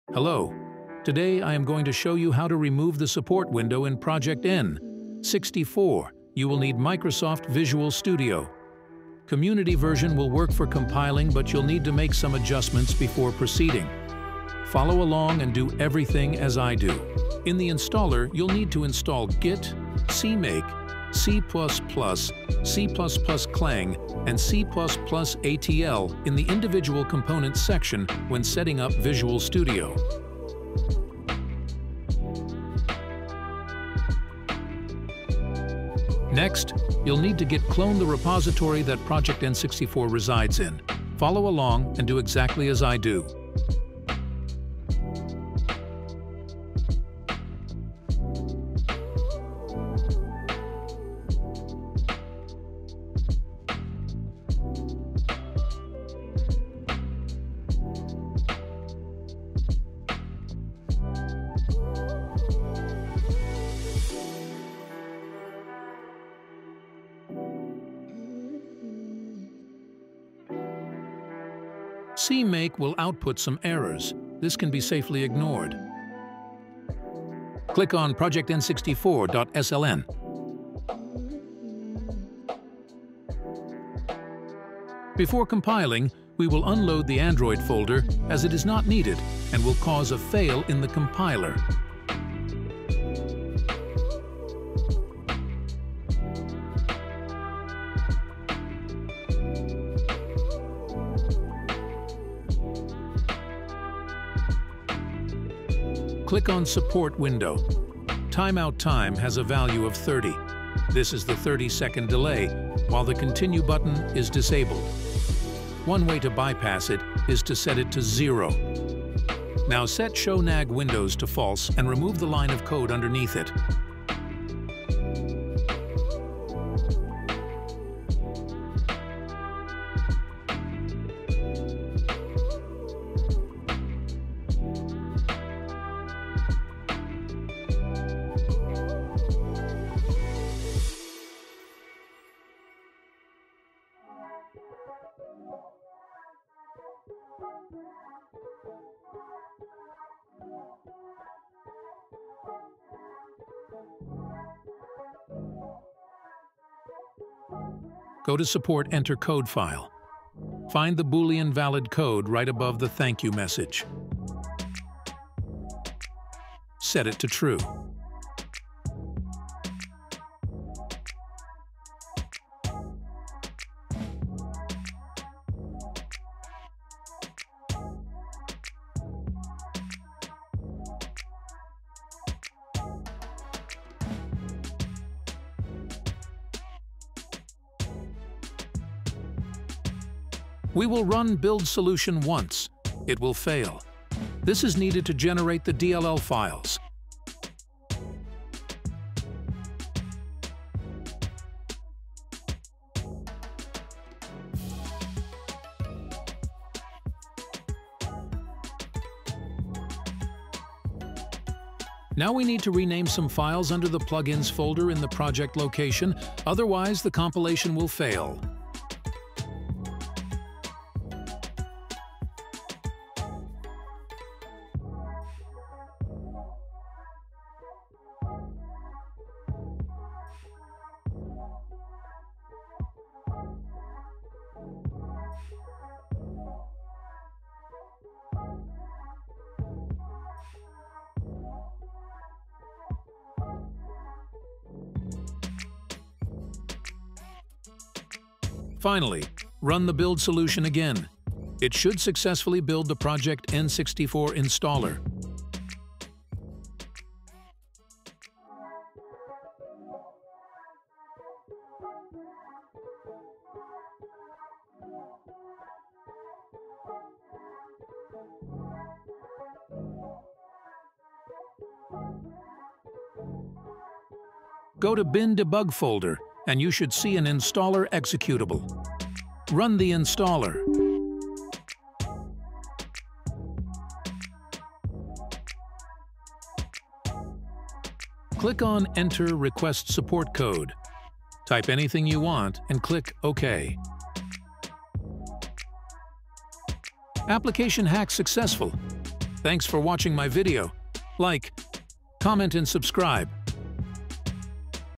How to remove that annoying support window in Project64! *New Method* Visual Studio Tutorial (2024)
Project64 is a great emulator but the devs force you to sit through a 30 second popup window nagging you to support the developers. This tutorial will show you how to bypass it!!